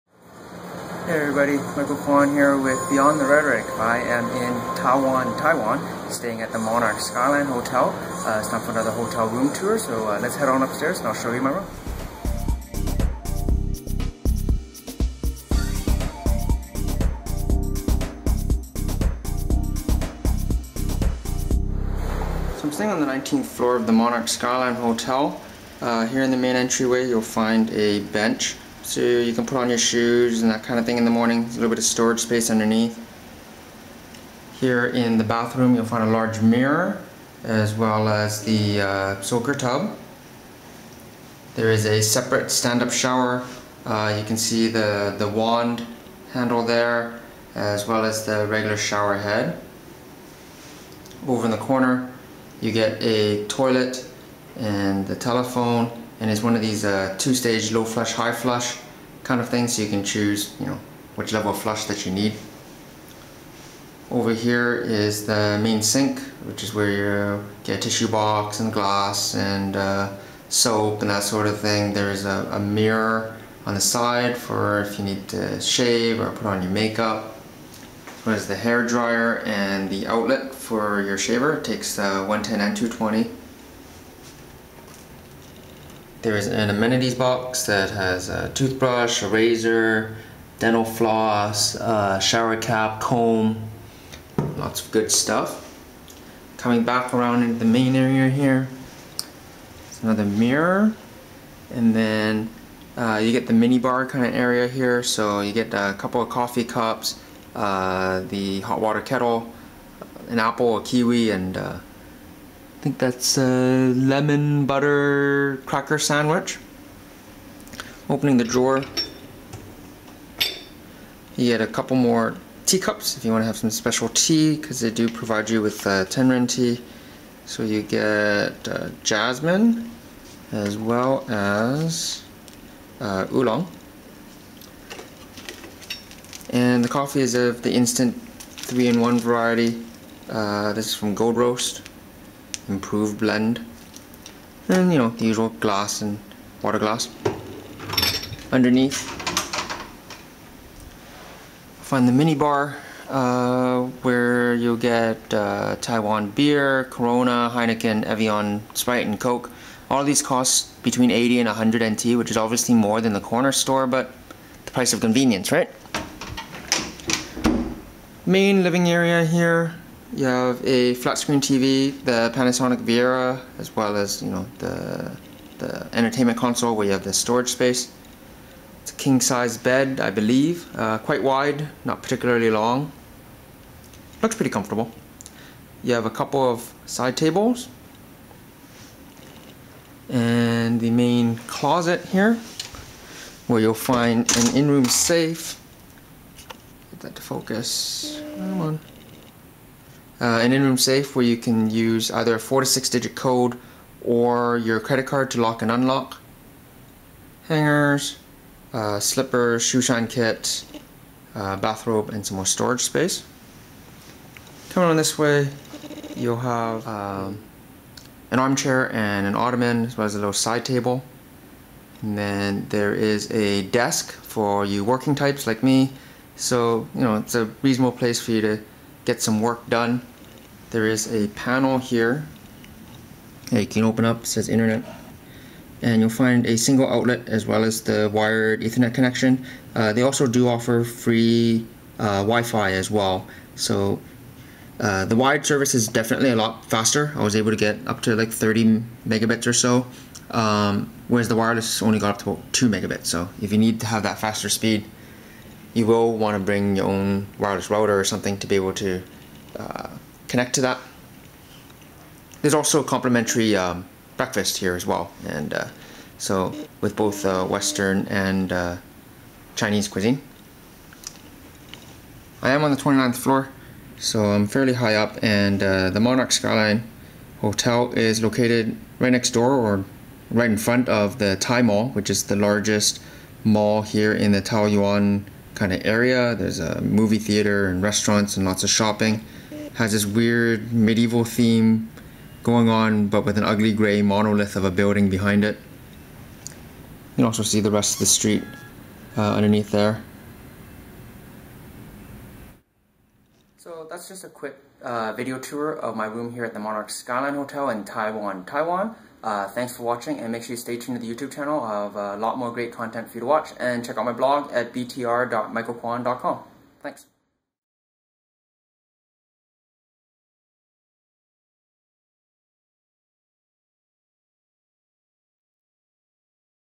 [HD] Monarch Skyline Hotel in Taoyuan, Taiwan
- A video tour of my hotel room at the Monarch Skyline Hotel in Taoyuan County, Taiwan. A relatively short drive from the main international airport in Taiwan.